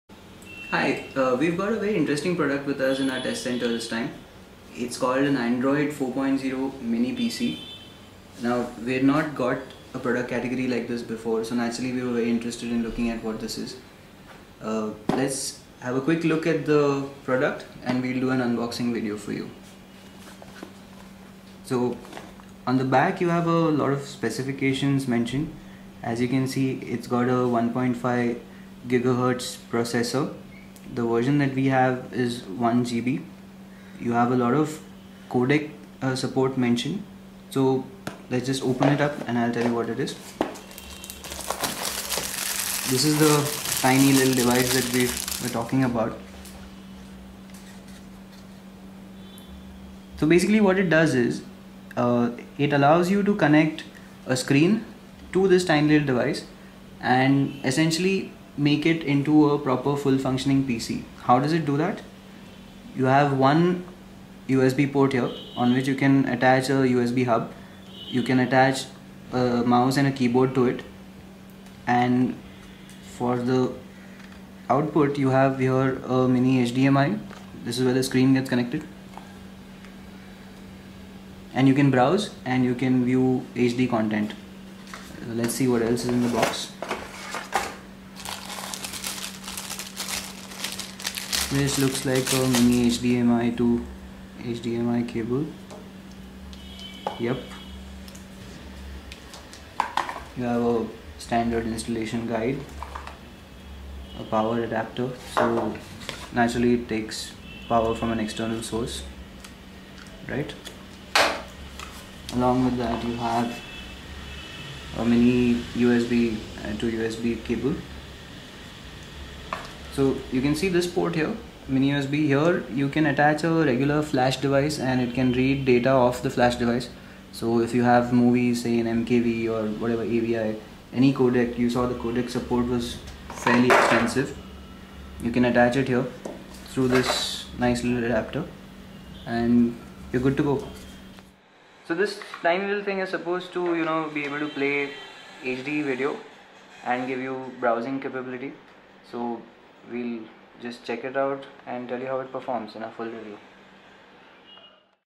Unboxing - Android 4.0 Mini PC
We check out the KY-108, or Android 4.0 Mini PC as its popularly called. It's a small Android device from Koway that lets you attach a screen, mouse and keyboard and use it for browsing and watching HD content. In a way, it lets you turn a regular TV into a Smart TV. Specs include - 1 GB DDR3 memory and an Allwinner A10/1.5 GHz Cortex A8 processor. In this unboxing video we check out the product and bundle.